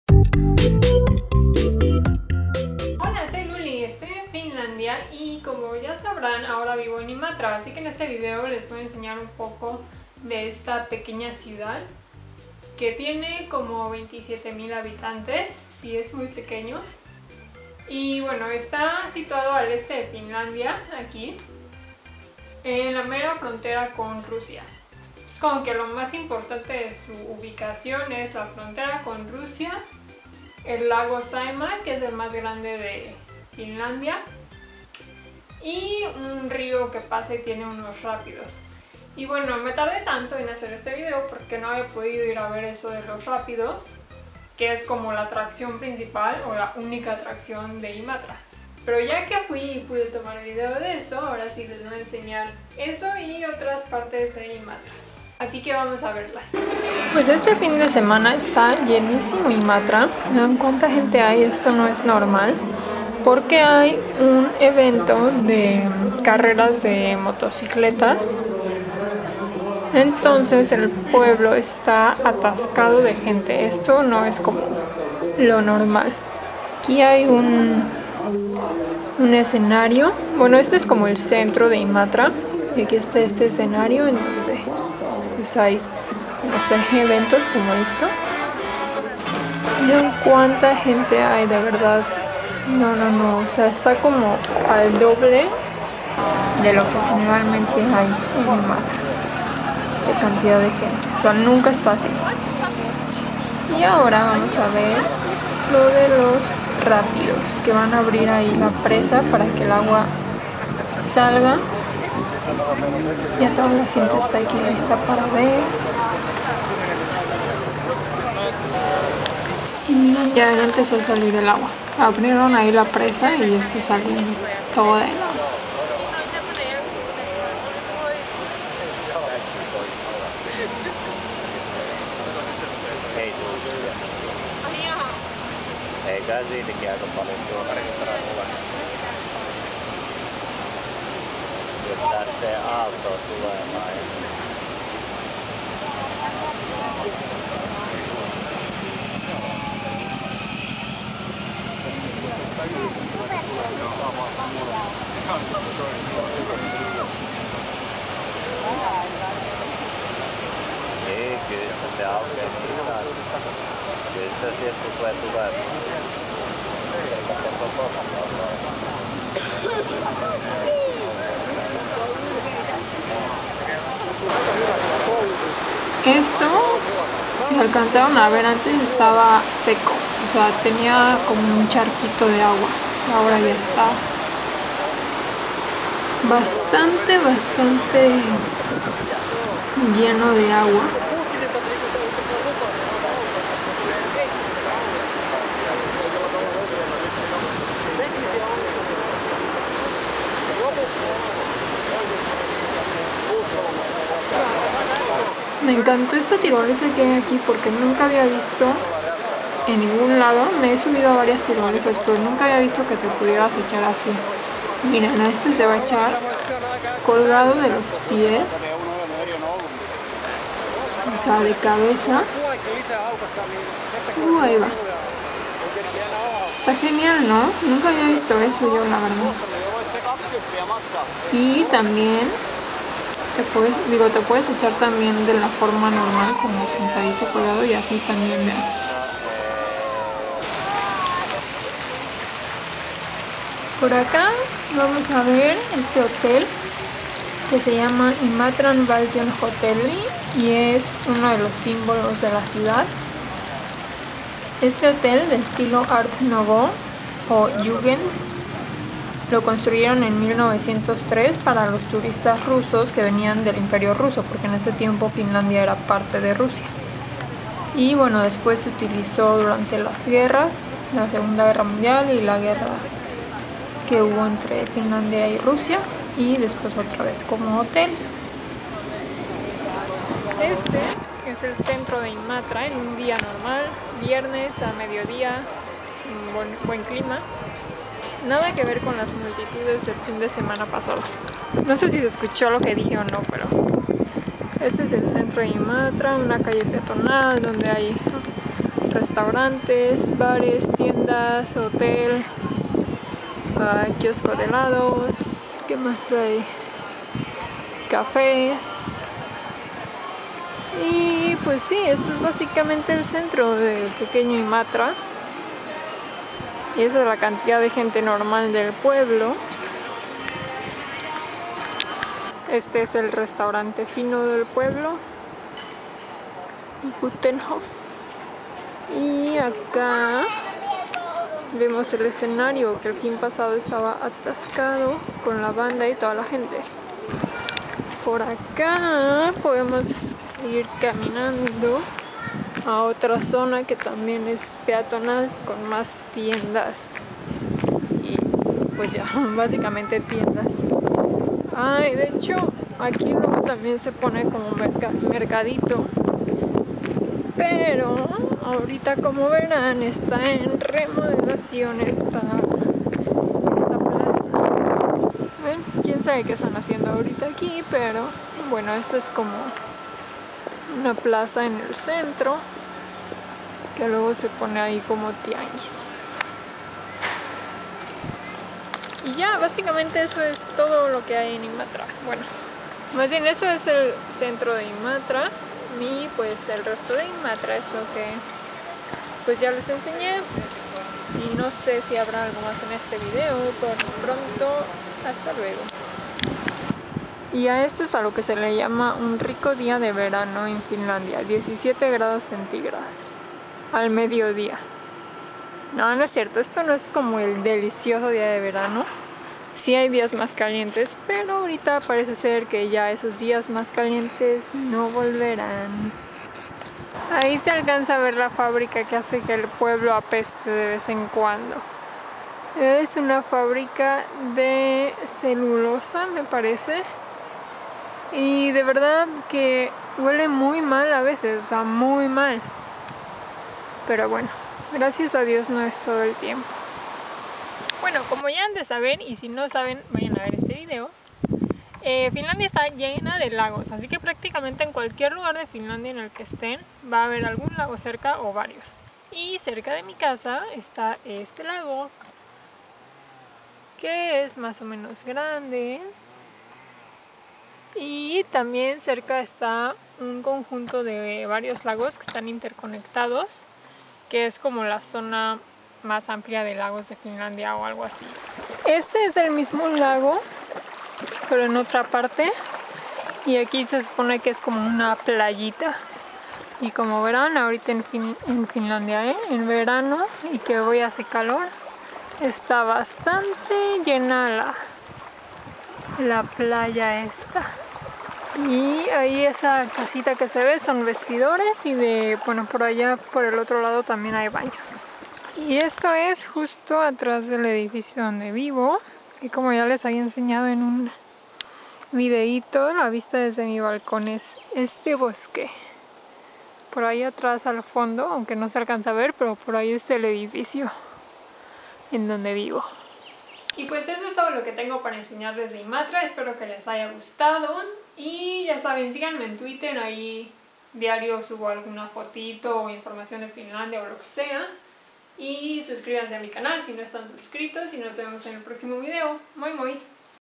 Conociendo Imatra, mi nuevo hogar | Luli en Finlandia
Por fin les tengo un video enseñándoles Imatra! Les muestro el centro, los rápidos, el Valtionhotelli, un lago y un bosque detrás de mi casa. Espero que les guste y que puedan así conocer y ver un poco más de Finlandia :)

Síganme en Twitter @luli_so

Mapa de Imatra
By Original artist: Ningyou. SVG-map created by Care - SVG map of Finland is created by user Care. Original bitmap map is created by User:Ningyou, Public Domain,